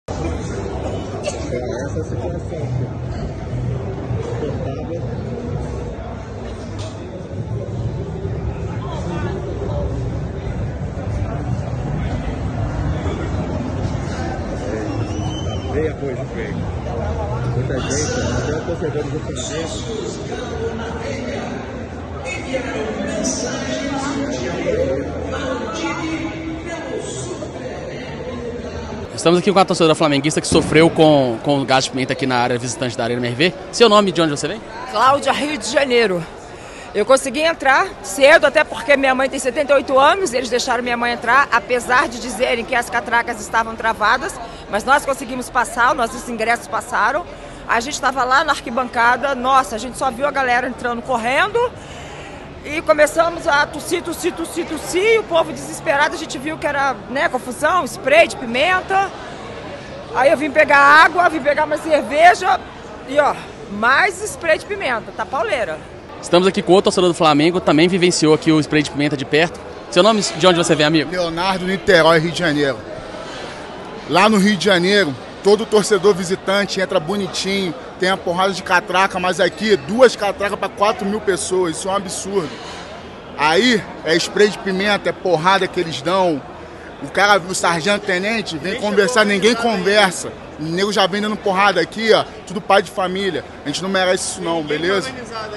ATLÉTICO X FLAMENGO: PÚBLICO DA ARENA MRV SENTE EFEITOS DE GÁS DE PIMENTA USADO FORA DO ESTÁDIO
Torcedores do Flamengo, presentes na Arena MRV para o jogo de volta da final da Copa do Brasil, neste domingo (10), às 16h (de Brasília), sofreram com gás de pimenta no processo de entrada no estádio. Segundo apurou a Itatiaia, houve um princípio de confusão no processo de revista antes da entrada dos flamenguistas no estádio.

A Polícia Militar de Minas Gerais (PMMG) usou gás de pimenta para dispersar flamenguistas durante o tumulto. No entanto, o gás afetou torcedores também que já estavam dentro da Arena MRV. Alguns torcedores e funcionários precisaram ser atendidos pela equipe de brigadistas.